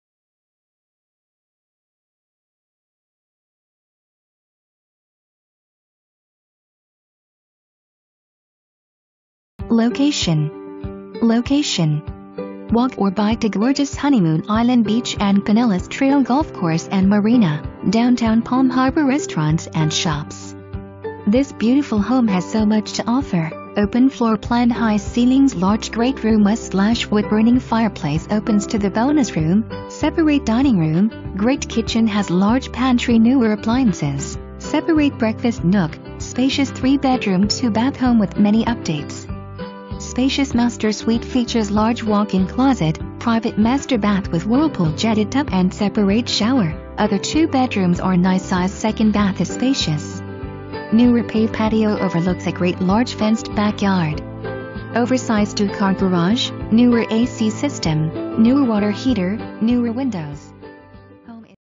240 WHISPER LAKE ROAD
240 WHISPER LAKE ROAD
$1,899, 3 bed, 2.00 bath, 1,584 SF, MLS# U8052526

Location! Location! Walk or bike to gorgeous Honeymoon Island Beach & Pinellas Trails Golf Course & Marina, downtown Palm Harbor restaurants & shops. This Beautiful home has so much to offer, Open Floor Plan high ceilings large great room w/ wood burning fireplace opens to the bonus room, separate dining room, great kitchen has large pantry newer Appliances, separate breakfast nook, spacious 3 bedroom 2 bath home with many updates. Spacious Master suite features large walk-in closet, private master bath with whirlpool jetted tub & separate shower, other two bedrooms are nice size 2nd bath is spacious. Newer Paved patio overlooks a great large fenced backyard.
Oversized 2 car garage, newer A/C system, newer water heater, newer windows. Home is located in Palm Harbor, close to Ozona, near Beaches, schools, shops, airports.  Don't miss out!

Luigi Kalaj
RE/MAX Realtec Group